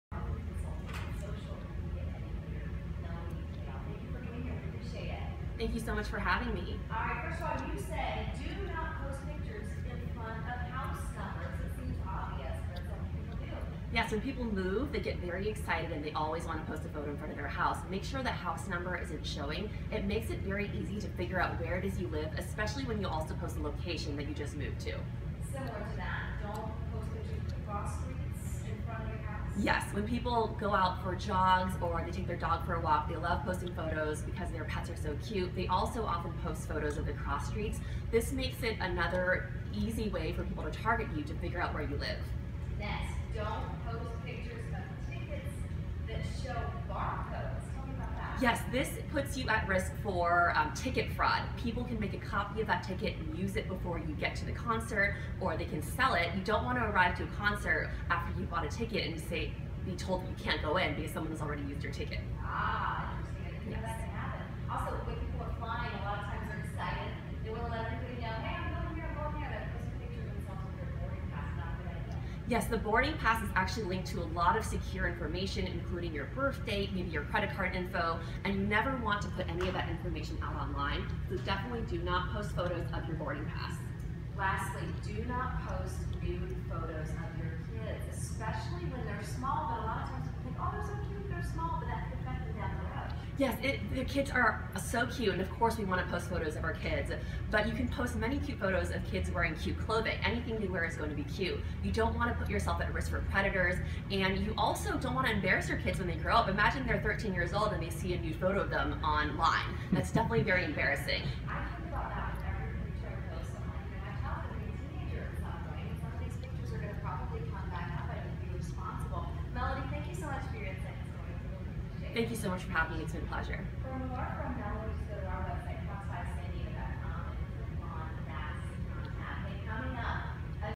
Behind the Scenes with Marketing Melodie on Fox 5 San Diego
Melodie Tao was featured on the Fox 5 San Diego morning news on April 19th, 2016 to discuss social media safety tips. Here's a behind the scenes clip of the segment airing live.